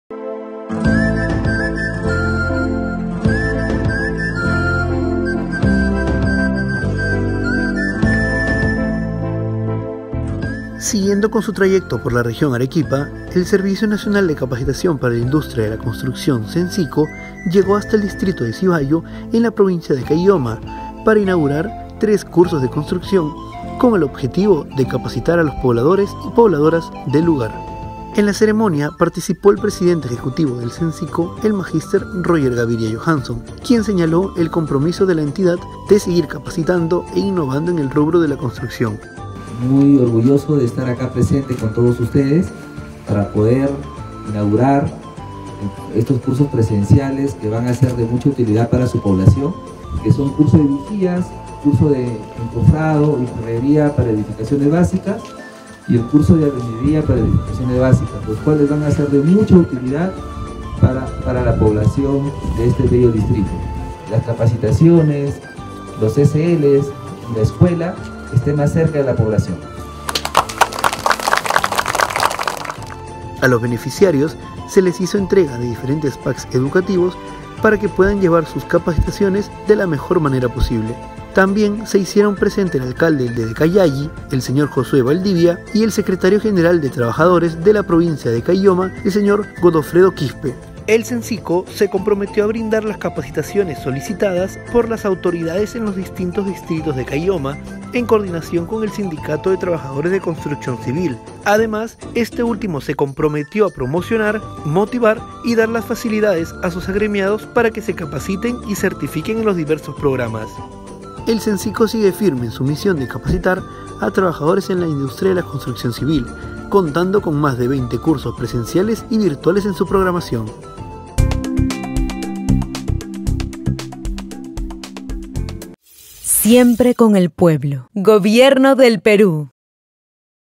SENCICOArequipa llegó hasta el distrito de Sibayo, Caylloma para inauguración de cursos.
[SENCICOArequipa] El presidente ejecutivo del SENCICO, Mg. Roger Gavidia, llegó hasta el distrito de Sibayo en la provincia de Caylloma para inaugurar 3 cursos de construcción, albañilería, vigía y Fierrería y Encofrados, para capacitar a los pobladores y pobladoras del lugar.👷‍♂️👷‍♀️

➡️En la ceremonia también participaron el Alcalde del distrito de Sibayo, Sr. Isaac Noa, el alcalde de Callalli, Sr. Josué Valdivia y el Secretario General de Trabajadores de la Provincia de Caylloma, Sr. Godofredo Quispe.🇵🇪